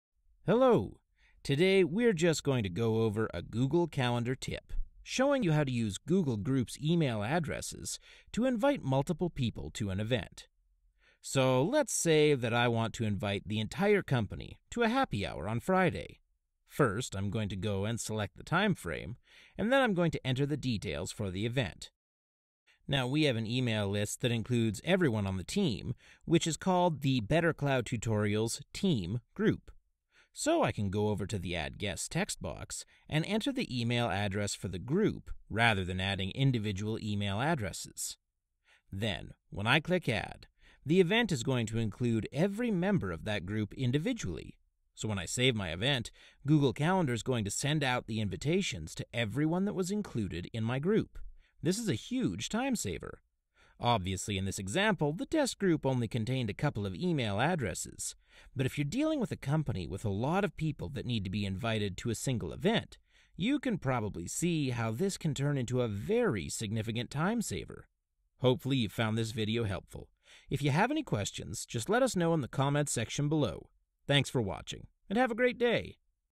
Inviting a Large Group of People to a Google Calendar Event
Have an event like a holiday party or office lunch where you need to invite a large group of people? Maybe the whole company? We’ll show you exactly how to do it in this video.